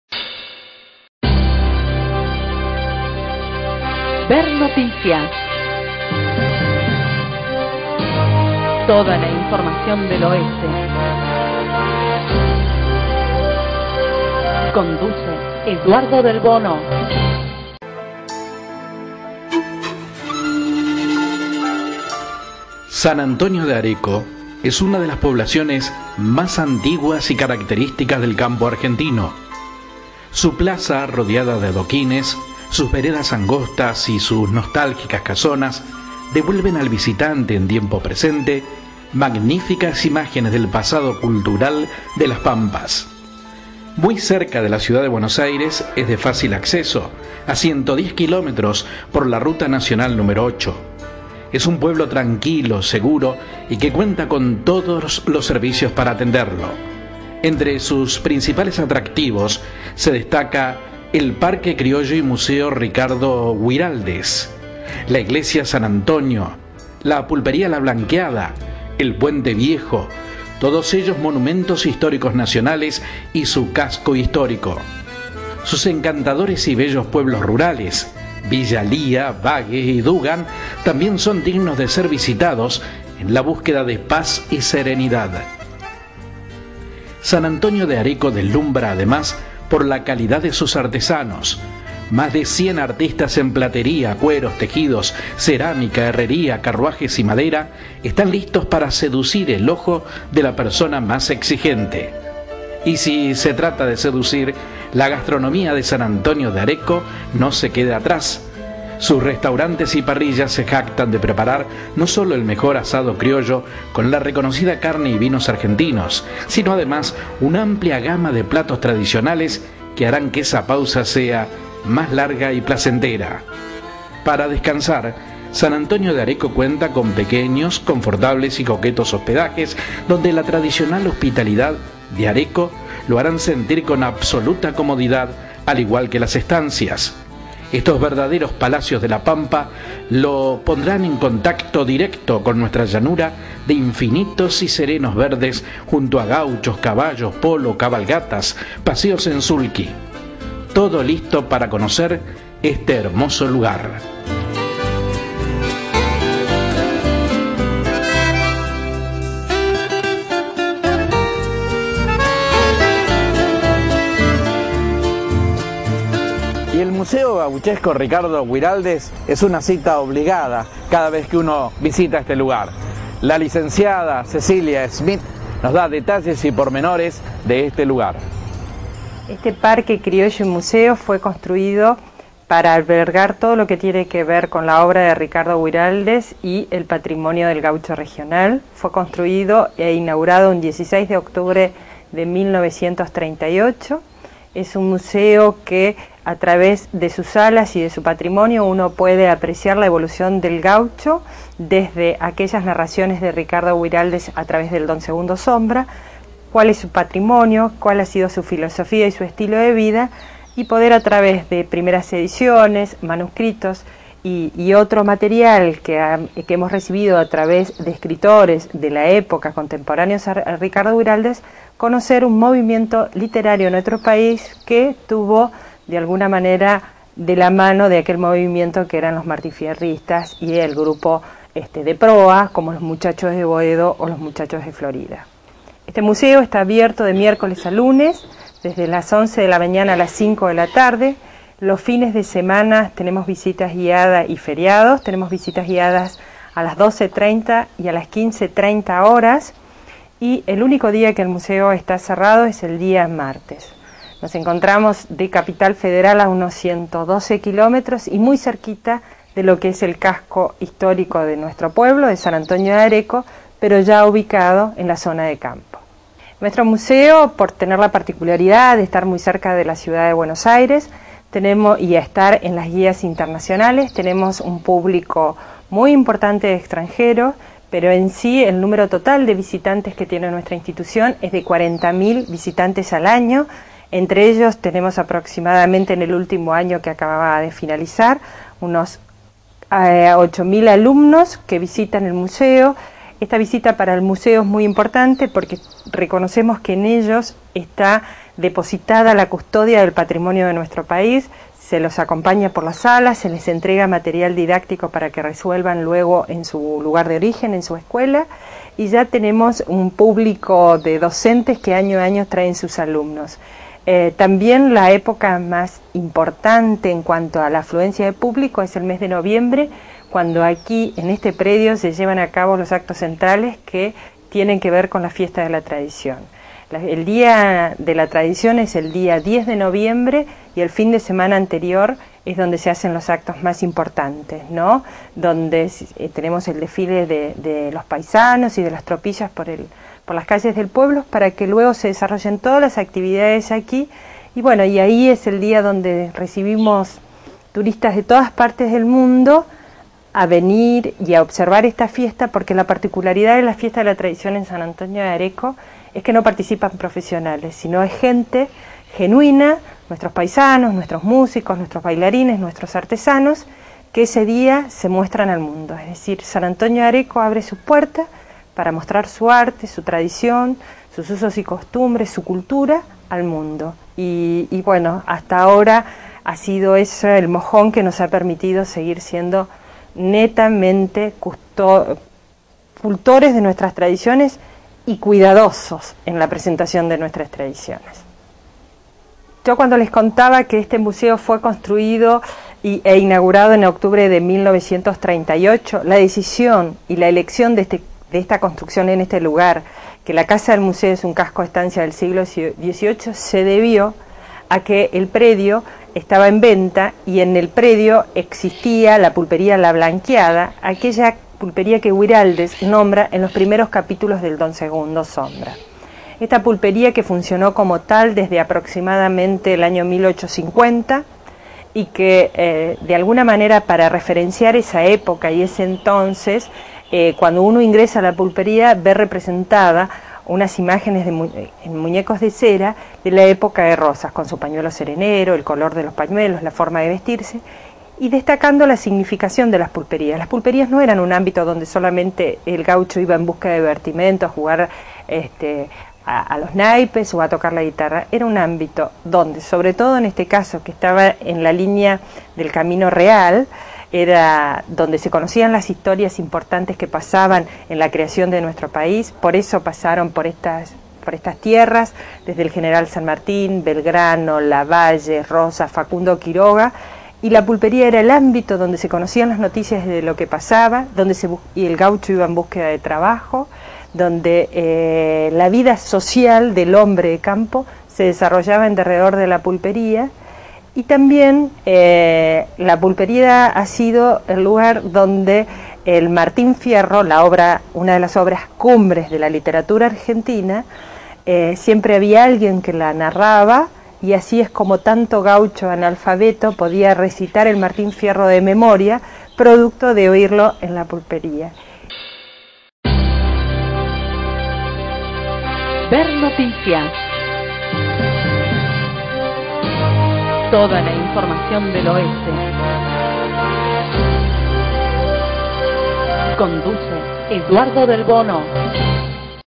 Visitamos San Antonio de Areco
Una realización audiovisual del periodista Eduardo Delbono para Ver Noticias
Canal 5 de Merlo, Pcia. de Bs.As.